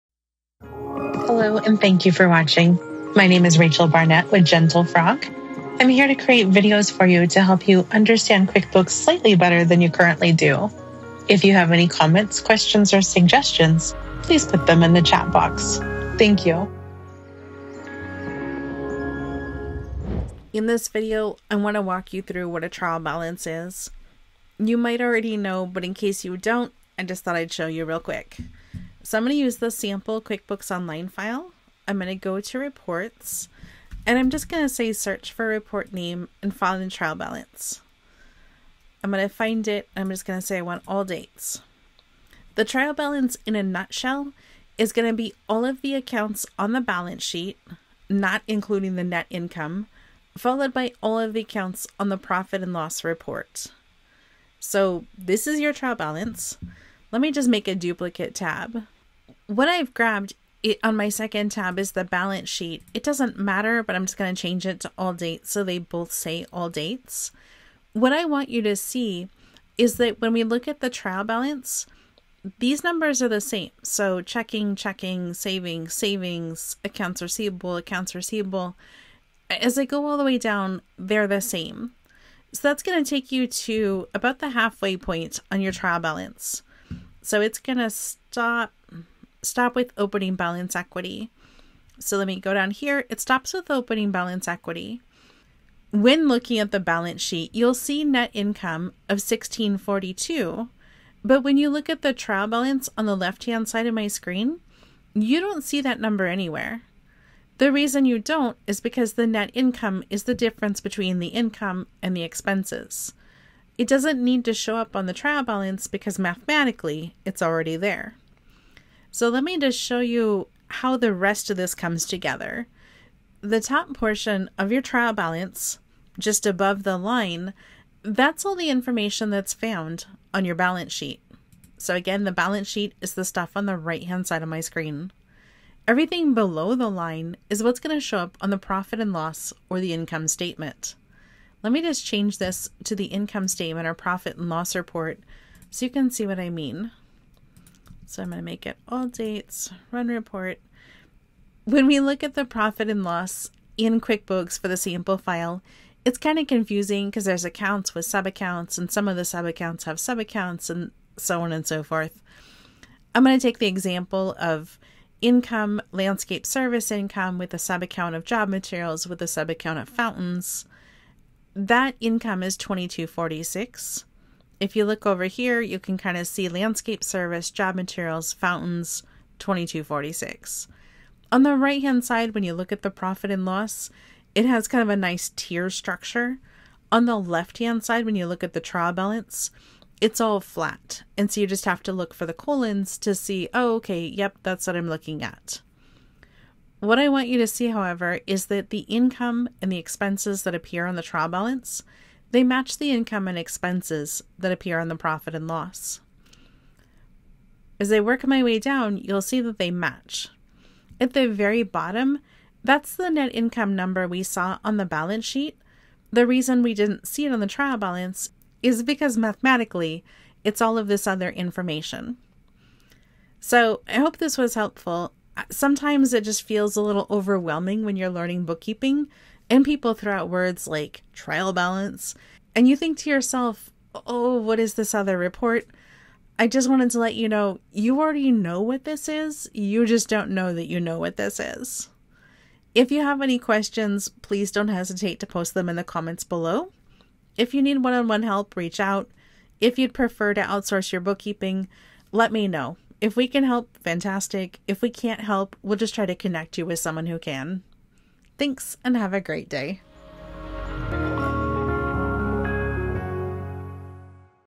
Understanding the Trial Balance Report in QuickBooks Online
Ever come across the term "trial balance" and wondered what it entails, especially within QuickBooks Online? This video is designed to demystify the Trial Balance report for you, whether you're familiar with the term or encountering it for the first time. Using a sample QuickBooks Online file, I'll guide you through what a trial balance is, how it's structured, and why it's an essential component of your financial reporting.

What You'll Learn:
- The Basics of a Trial Balance: An introduction to what a trial balance is and its significance in accounting and bookkeeping.
- Comparison with Balance Sheet and Profit & Loss: How the trial balance relates to other crucial financial reports such as the Balance Sheet and the Profit & Loss statement, highlighting the similarities in the figures presented.
- Understanding the Structure: The composition of a trial balance, dividing the accounts from the Balance Sheet and Profit & Loss report, and why the net income doesn't appear on the trial balance.
- Navigational Tips in QuickBooks Online: Step-by-step instructions on how to access the trial balance report in QuickBooks Online and how to interpret the data it presents.

This tutorial is aimed at small business owners, bookkeepers, and anyone looking to deepen their understanding of financial reports within QuickBooks Online. By the end of this video, you'll not only grasp what a trial balance is but also appreciate its role in encapsulating your business's financial standing.

Timestamps
00:00 - Intro
00:28 - Navigate to the Trial Balance report
00:42 - What is the Trial Balance
00:58 - Trial Balance vs Balance Sheet
02:30 - Trial Balance vs Profit & Loss
04:00 - Outro